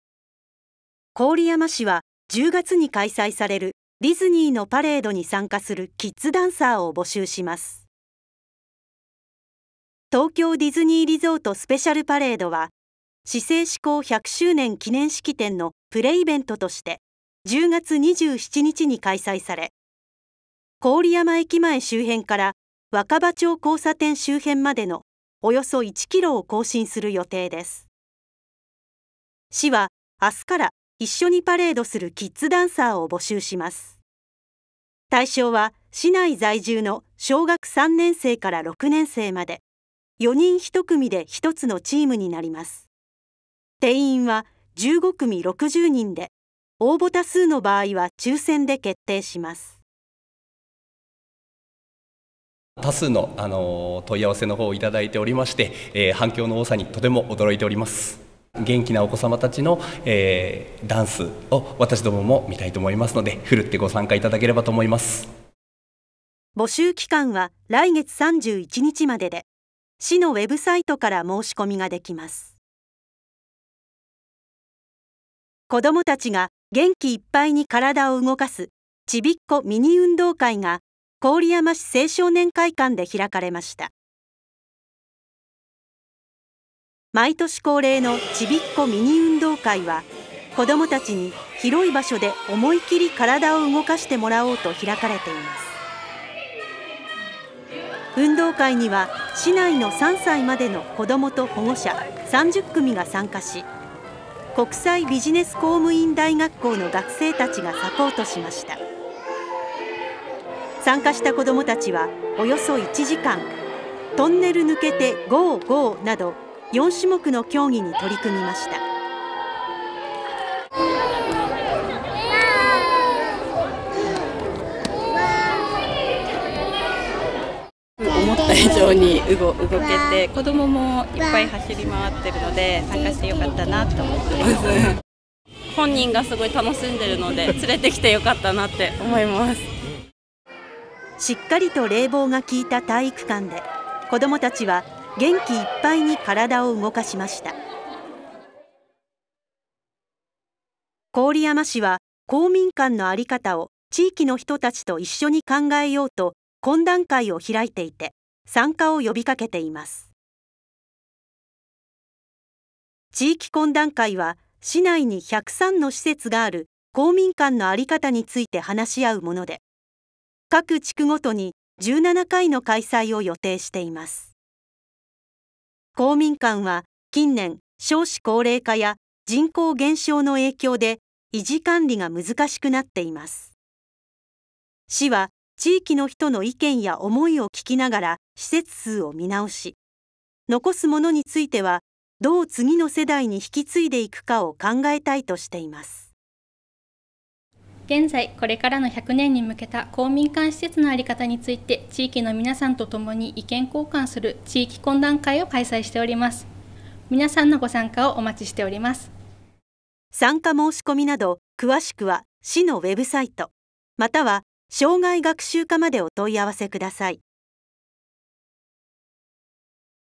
【郡山市週間トピックス】2024/6/30放送
ディズニーのパレード キッズダンサーを募集/郡山市青少年会館でちびっこミニ運動会/公民館のあり方を考える地域懇談会の参加者募集